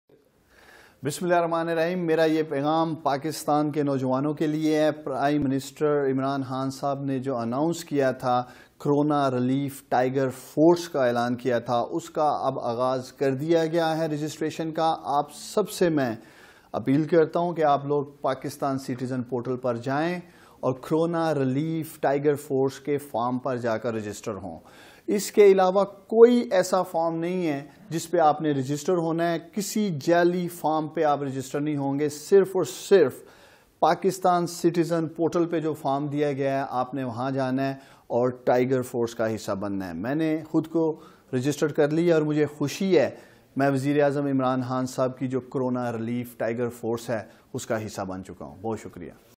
عالمی وباء سے جنگ میں اپنے غریب طبقے کا مددگار بننے کیلئے *وزیراعظم کورونا ریلیف ٹائیگرز
عالمی وباء سے جنگ میں اپنے غریب طبقے کا مددگار بننے کیلئے *وزیراعظم کورونا ریلیف ٹائیگرز فورس* کی رجسٹریشن کا باقاعدہ آغاز کر دیا گیا ہے ملک بھر کے نوجوان صرف اور صرف پاکستان سیٹیزن پورٹل کے ذریعے ہی رجسٹر ہو سکتے ہیں، خود کو ٹائیگر فورس میں رجسٹر کرانے پر پرجوش ہوں۔